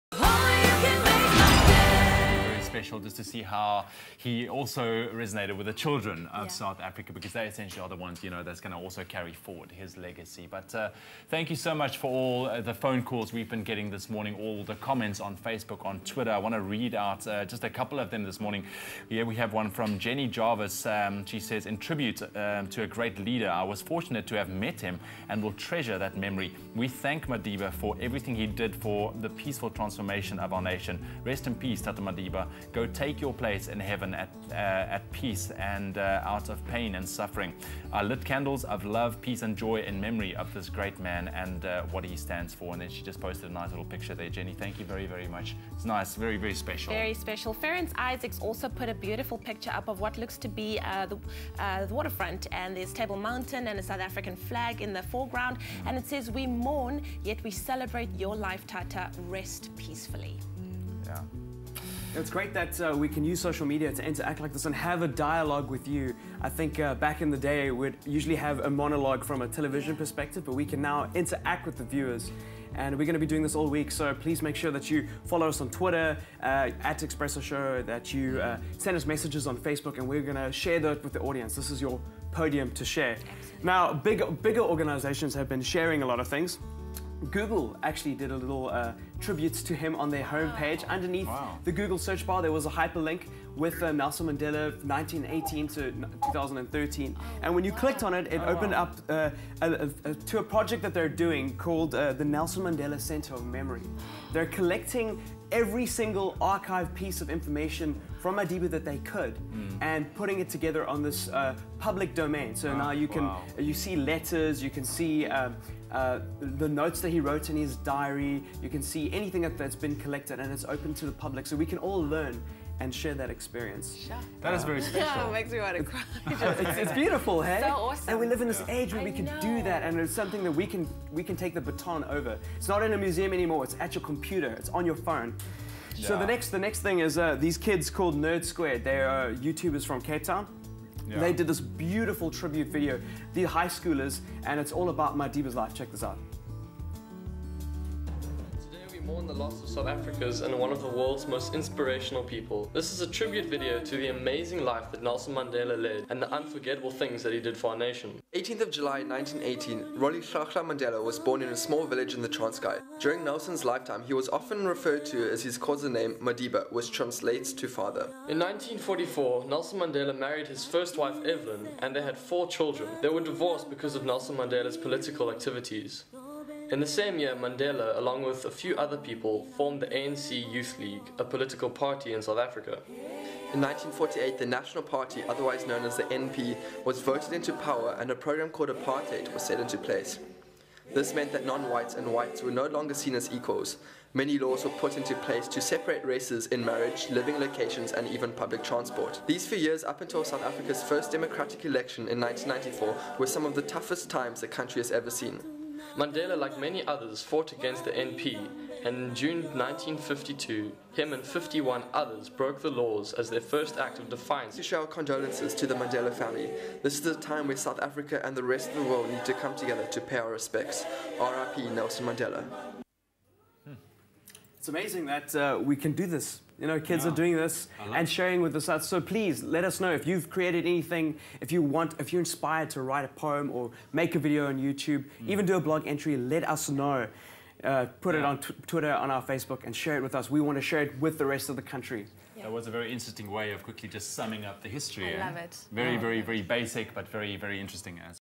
Social Media Report: Mandela Tributes
Grant hinds shows us how social media has been a huge contribution to Mandela's tributes.